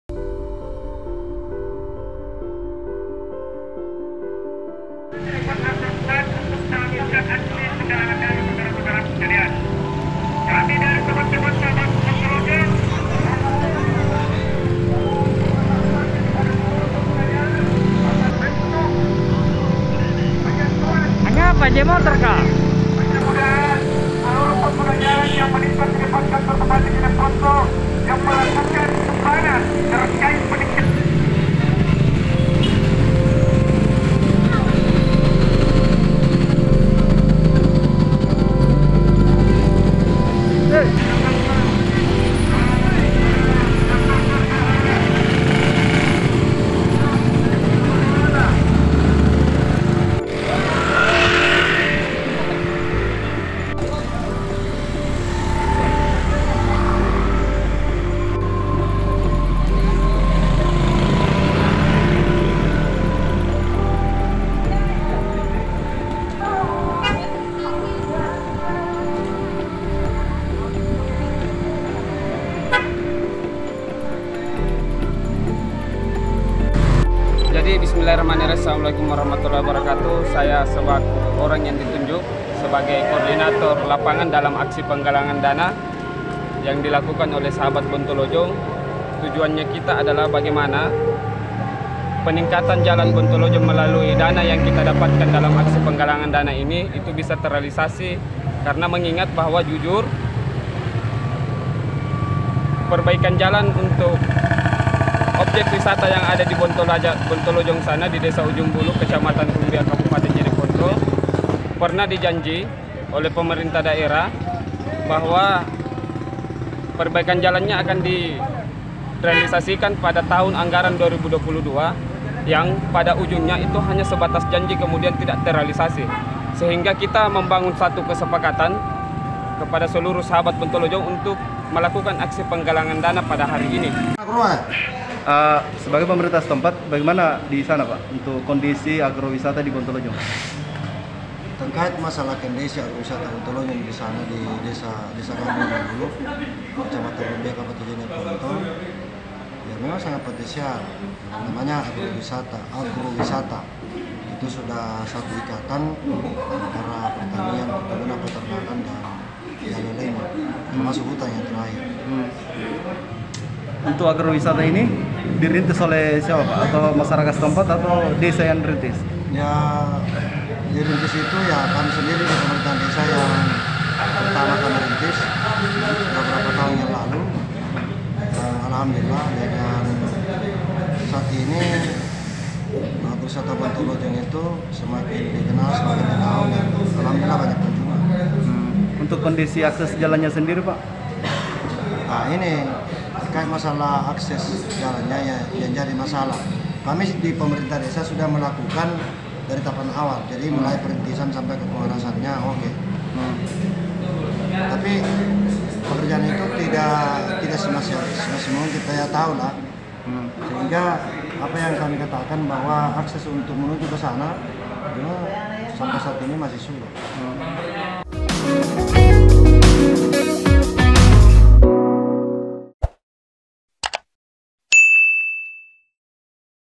Akses Jalan Menuju Agro Wisata Bontolojong Rusak Parah, Warga Gelar Aksi Galang Dana
Puluhan warga Jeneponto yang tergabung dalam ‘Sahabat Bontolojong’ menggelar aksi penggalangan dana perbaikan akses jalan Agrowisata wisata Bontolojong di Desa Ujung Bulu, Kecamatan Rumbia.

Aksi Galang dana ini digelar di depan Kantor Bupati Jeneponto, Jalan Lanto dg Pasewang dan Depan Kantor DPRD Jeneponto, Jalan Pahlawan, Kamis (12/05/2022).

Video terkini dan terbaru..